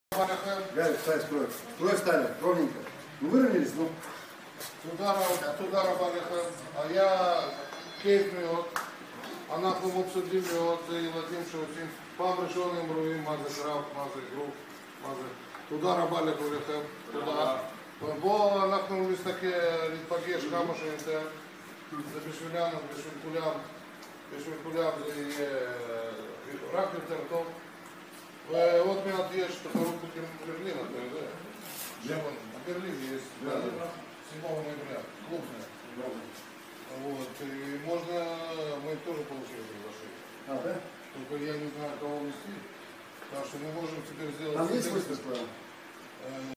Mazirov center Boxing Israel
דמיטרי מזירוב מהמן איגרוף קלאסי ב רמת גן.